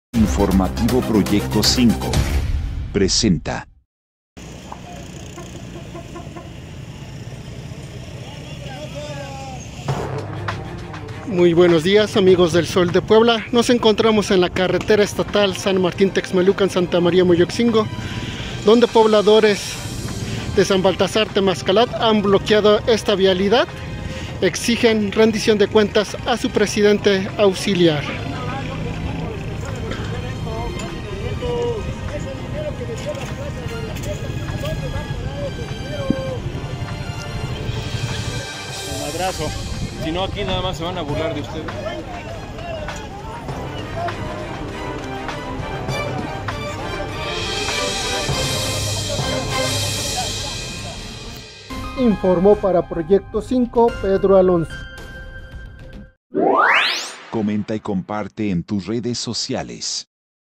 Con bloqueo de la Carretera Texmelucan-Moyotzingo exigen cuentas a subalterno de Temaxcalac
Se bloquea la carretera estatal San Martín Texmelucan-Santa María Moyotzingo, por pobladores de San Baltazar Temaxcalac, exigen al presidente auxiliar informe sobre los ingresos y egresos que tuvo al frente de la presidencia auxiliar de dicha comunidad